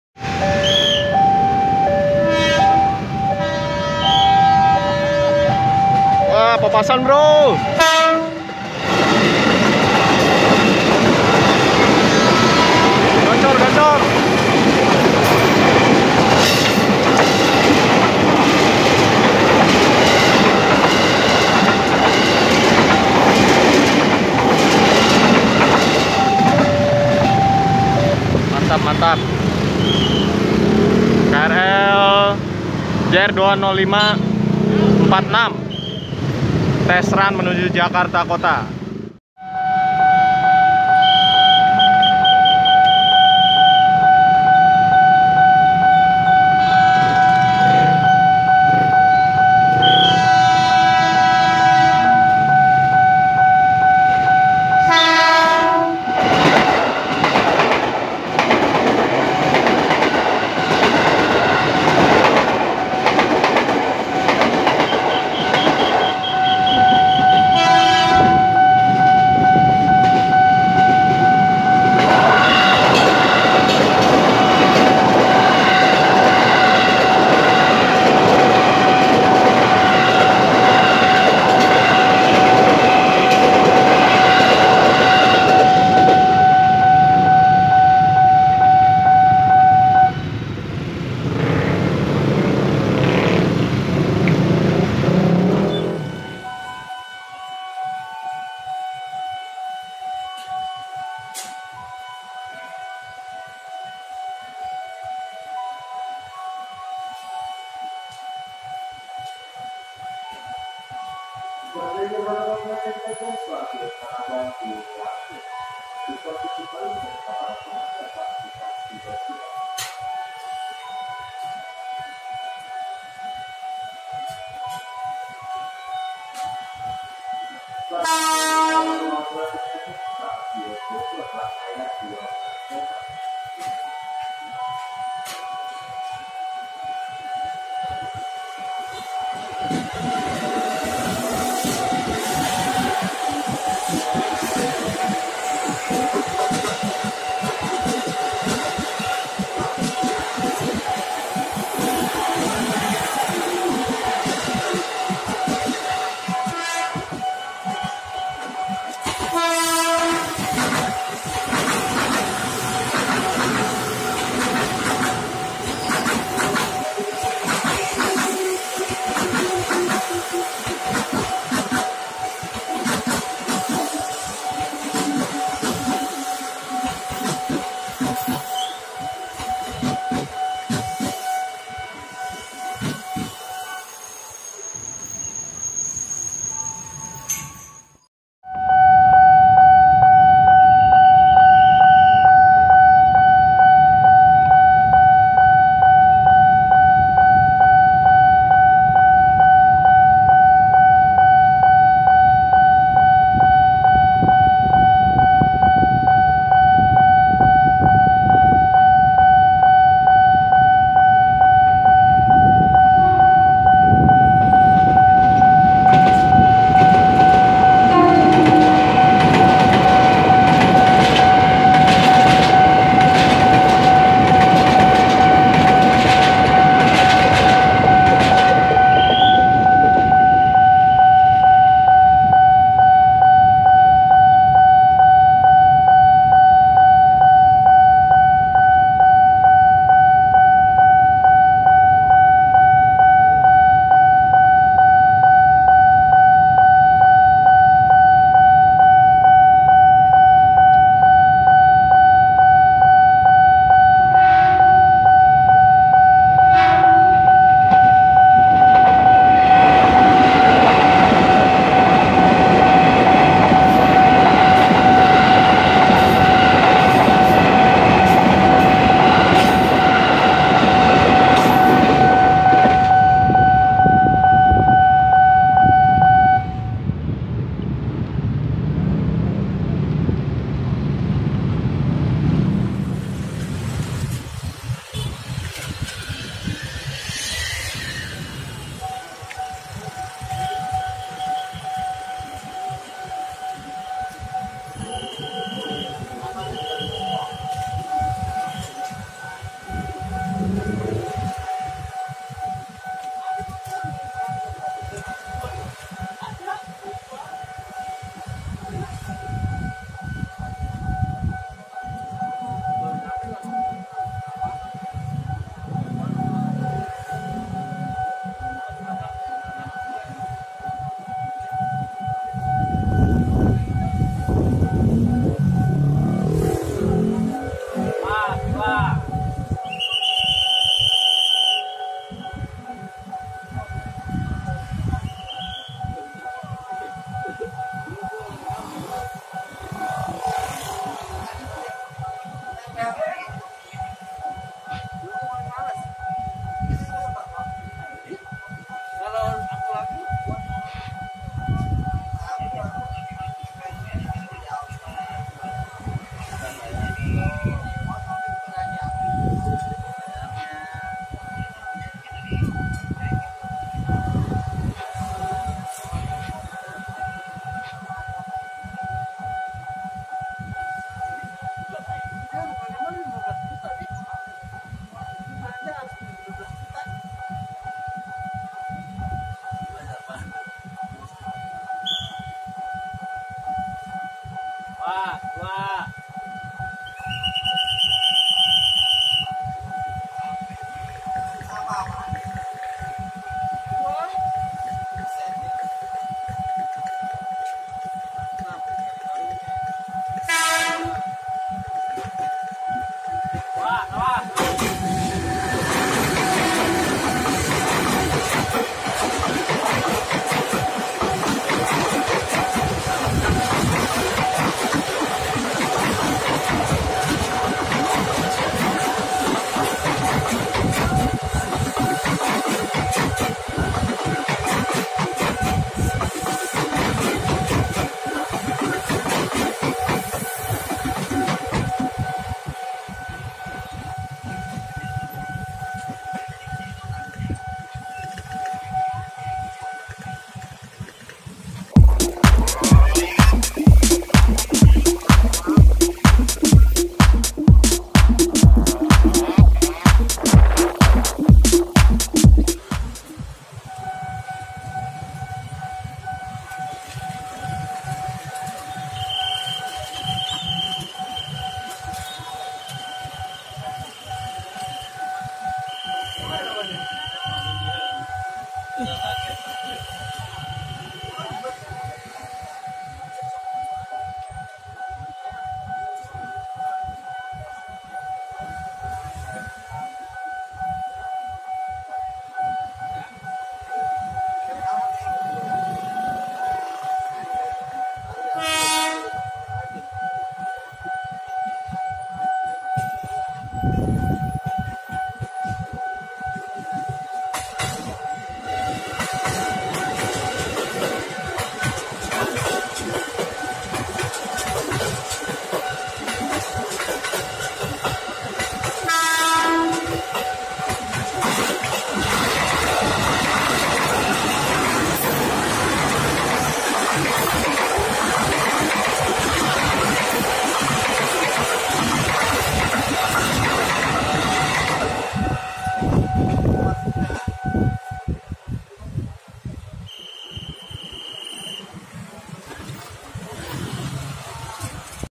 Railroad Crossing Indonesia Spesial Kompilasi KRL Baru Livery Kereta Listrik Commuterline Jabotabek
berikut saya buat kompilasi All new KRL Dengan Striping Baru Papasan dengan livery lama